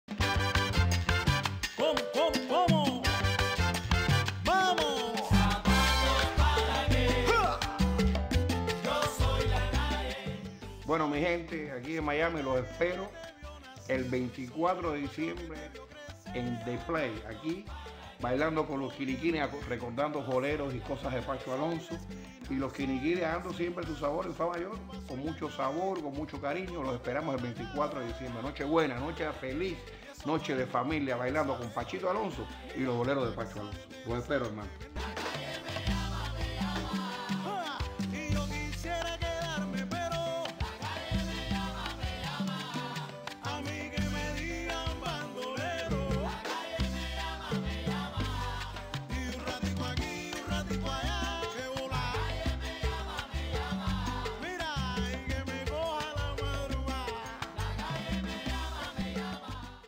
PROMO: PACITO ALONSO Y SUS KINIKINI
PACHITO ALONSO, de gira por Estados Unidos con presentación día 24 diciembre 2015 en The Place OF Miami.
Promo: alex_prod